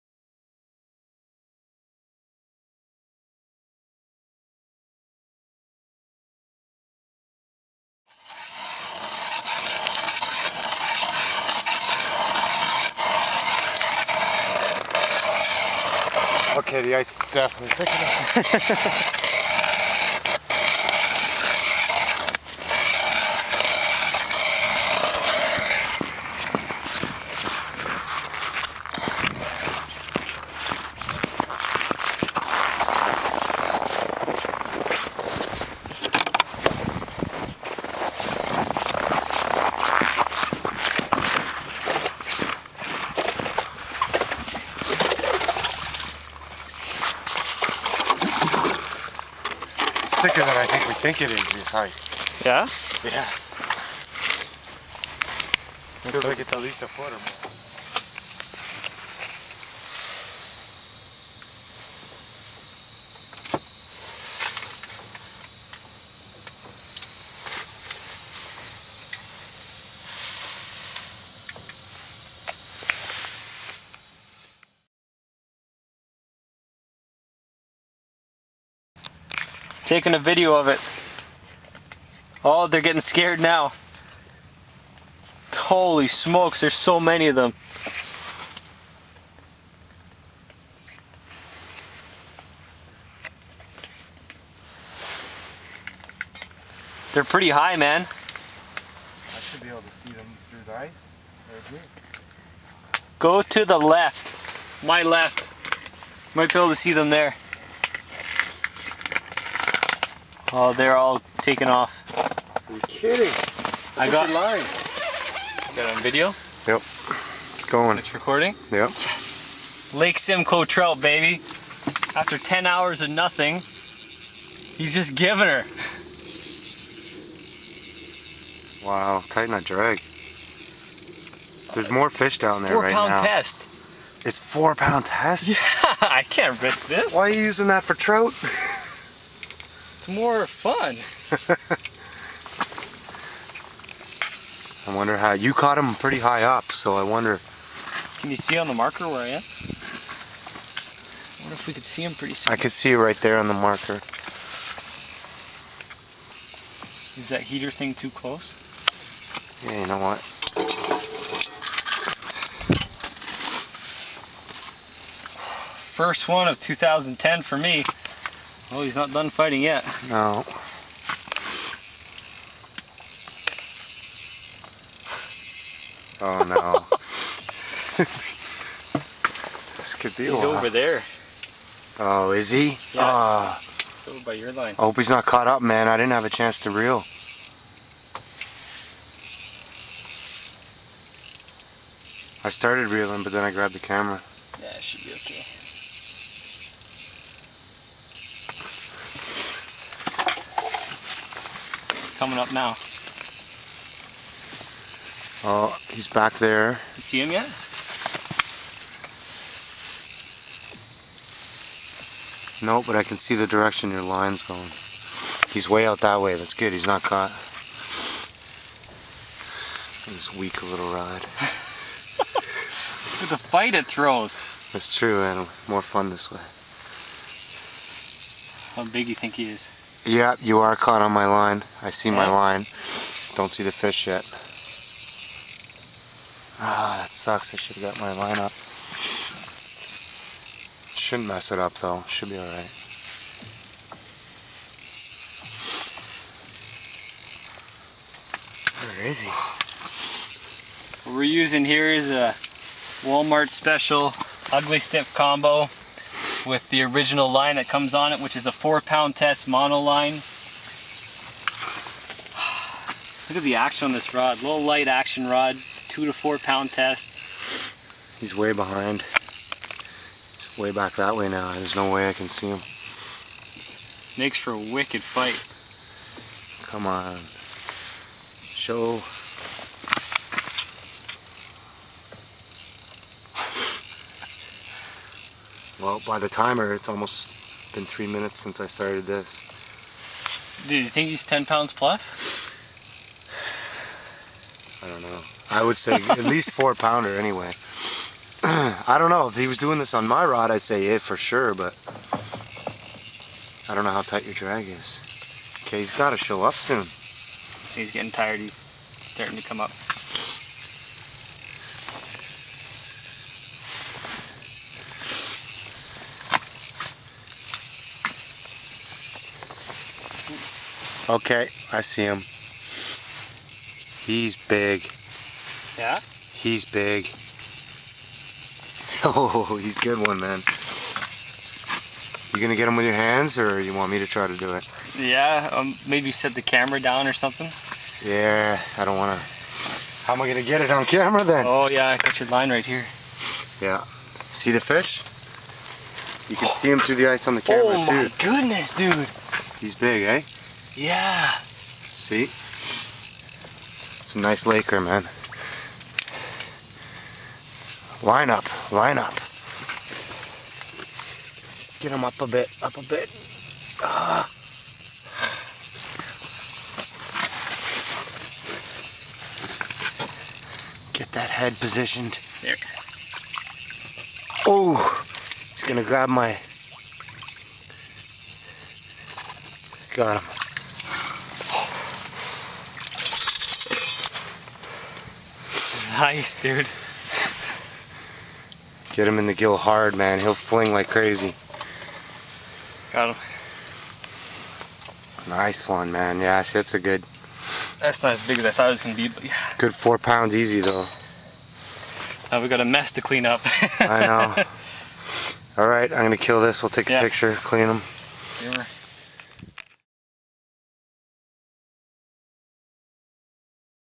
BIG LAKE TROUT ON SIMCOE!!
Please FAV and LIKE it helps me out a lot!
Catching a decent 5 lbs trout on Lake Simcoe. Also getting some footage of a school of fish going by! So awesome!

Film equipment:
GoPro Frame
GoPro Accessories

Other Gear
Editing software
Camera Case